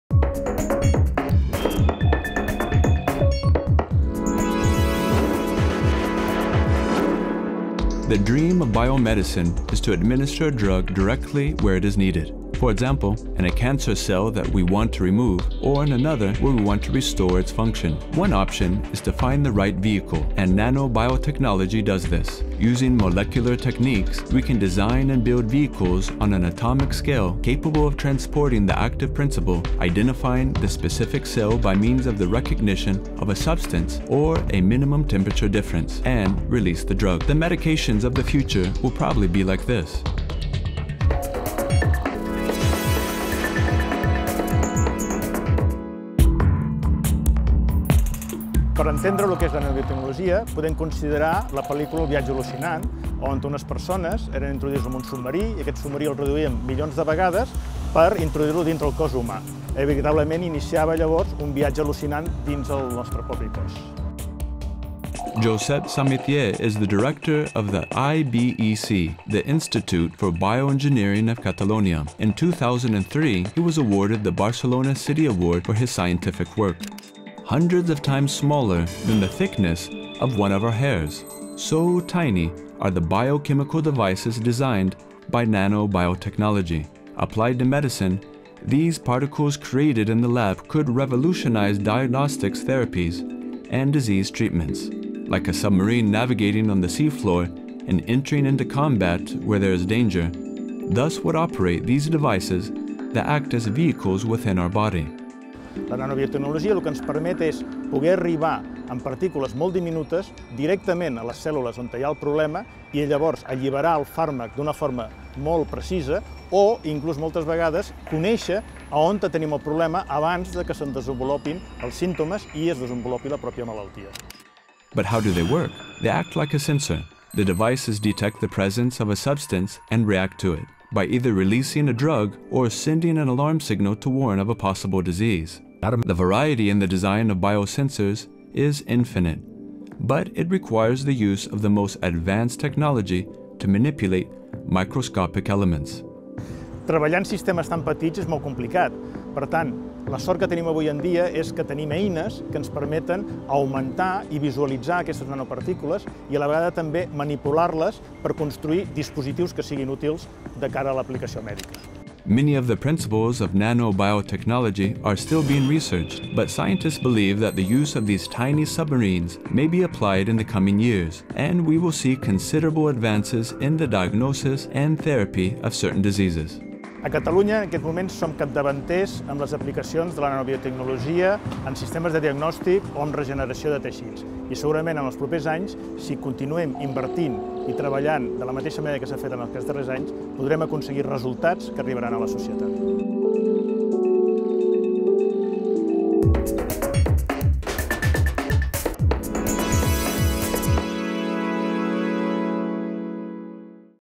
Science Pills. Josep Samitier (English)
Josep Samitier is director at IBEC, the Institute of Bioengineering of Catalonia. In 2003, He won the 'Ciutat Barcelona' award for his scientific research.

FANTASTIC VOYAGE
The dream of biomedicine is to administer a drug directly where is needed. For example, in a cancer cell that wish to remove or otherwise where we want to restore its function. One option is to find the right vehicle, and nanobiotechnology, it does. Using molecular techniques to design and build atomic scale vehicles capable of carrying the active principle, identify the specific cell through recognition of a substance or a minimum temperature difference, and release the drug. Probably, is what will do the drugs of the future.